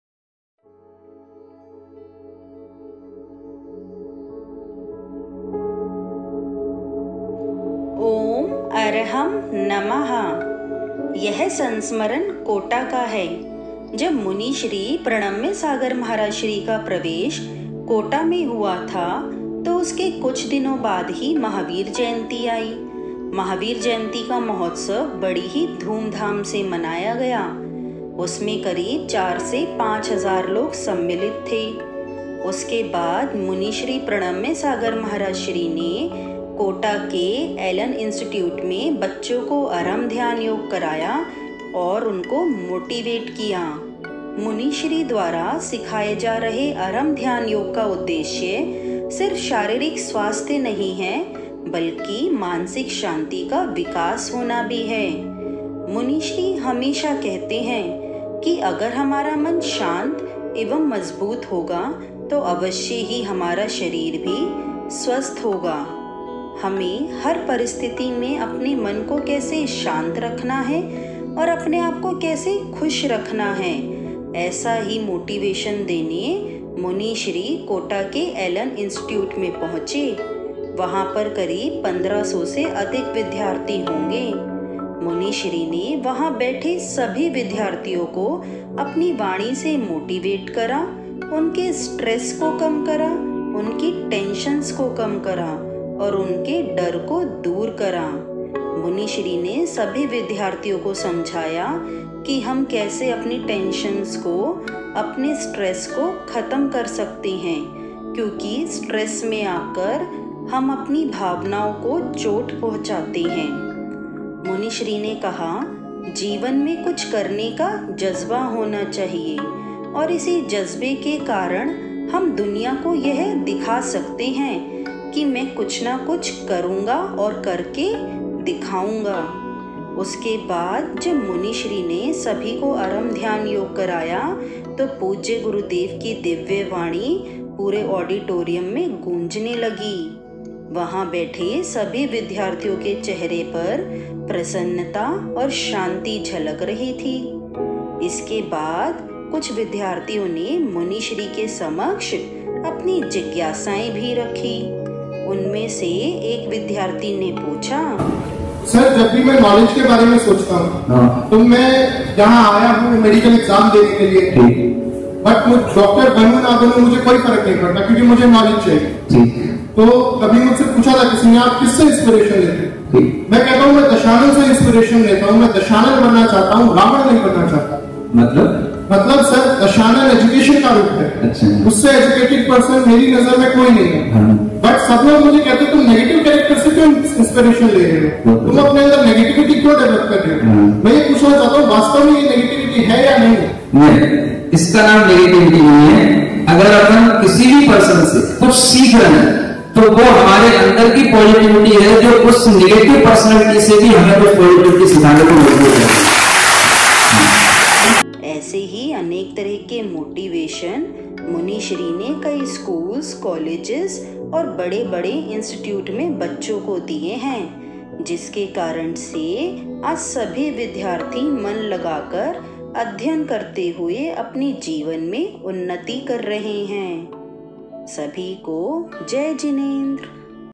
श्री प्रणम्य गुरु संस्मरण 13 | Allen University के बच्चों को कैसे किया Motivate
श्री प्रणम्य गुरु संस्मरण 13 | Allen University के बच्चों को कैसे किया Motivate | BhaktKiBhakti | Shri Pranamya Guru Sansmaran
🌸🌸 आइए सुनते हैं कि ALLEN INSTITUTE के बच्चों को मुनि श्री 108 प्रणम्य सागर जी महाराज ने कैसे मोटिवेट करा और उनके प्रश्नों का कितना अद्भुत उत्तर दिया।🌸🌸